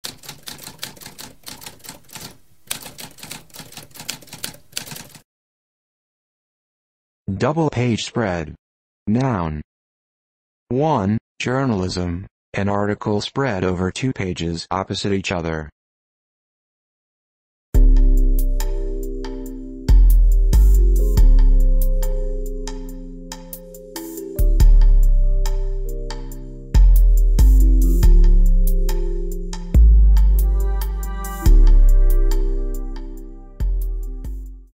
What does double-page spread mean?
A spoken definition of double-page spread.

Intro Sound:
Typewriter - Tamskp